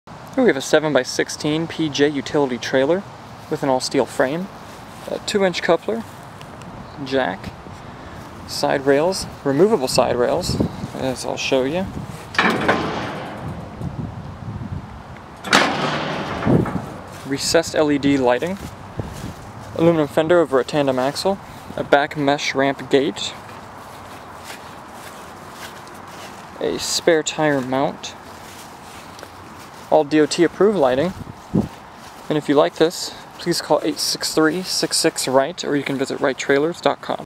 7x16 PJ Trailers | Utility Trailer
863-66-Right
7220 US Highway 98 North
Lakeland, Florida

All Steel Frame
Tandem Axle
Aluminum Fenders
2-3,500# (Dexter) One Electric / Spring
Straight Deck w/4' Fold-up Gate
Black Powder Coat
Spare Tire Mount
Wooden Deck
Stake Pockets
2" Coupler